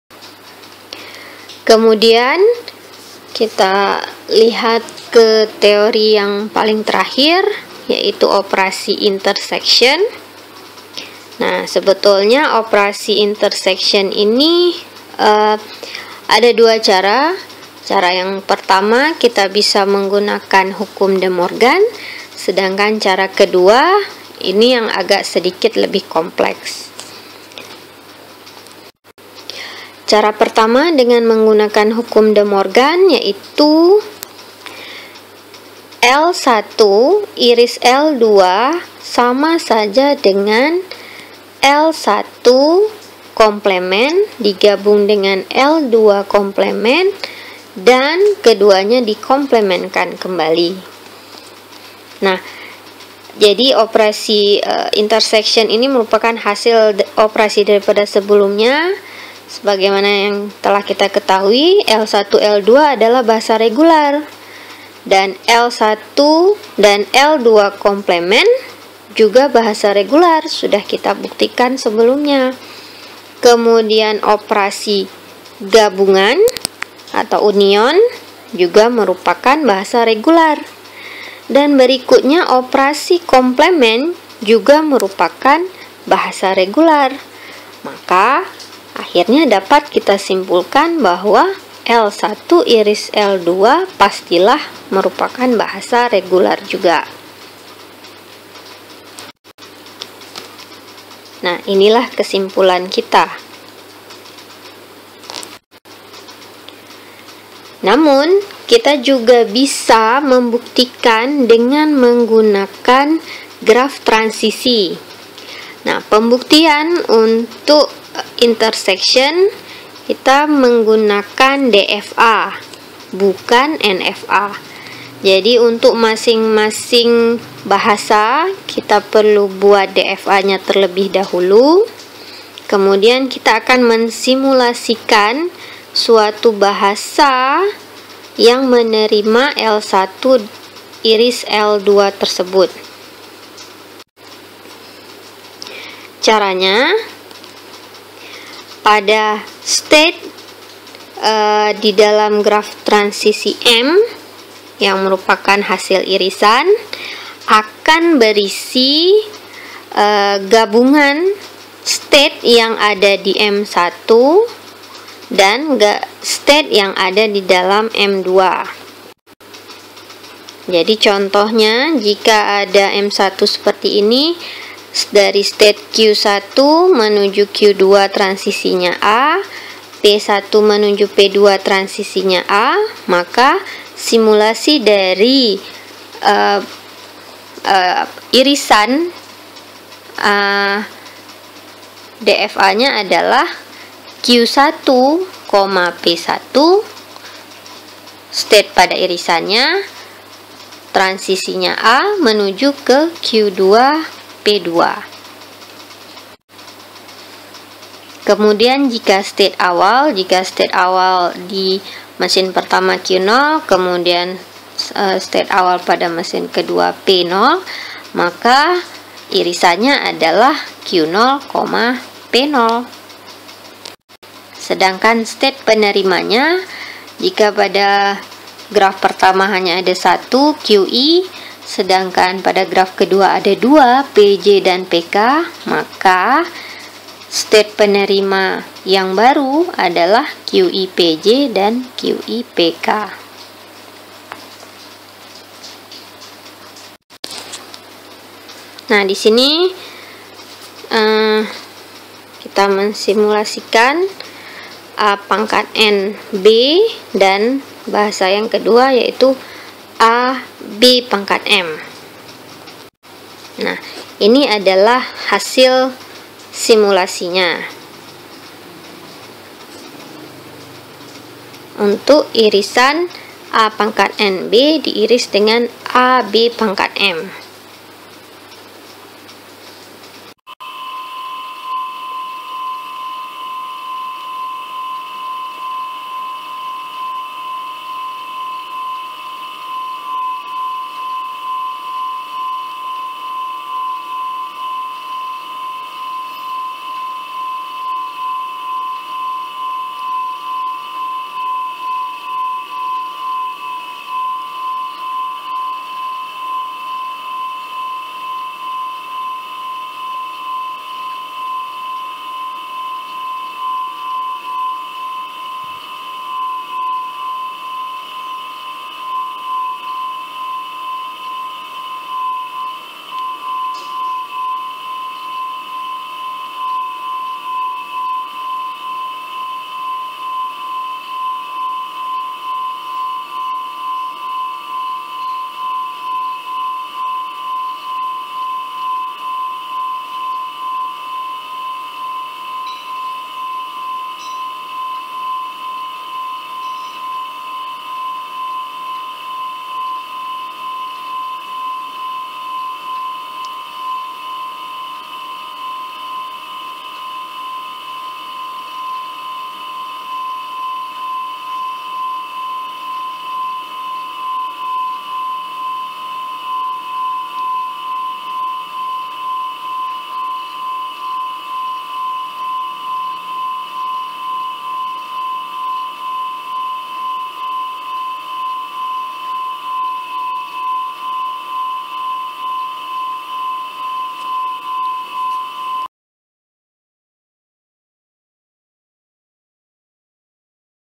Properti regular 4 automata
Video untuk Topik 5 - Properti Regular
Mata kuliah Teori Bahasa dan Automata
Jurusan Informatika
Universitas Syiah Kuala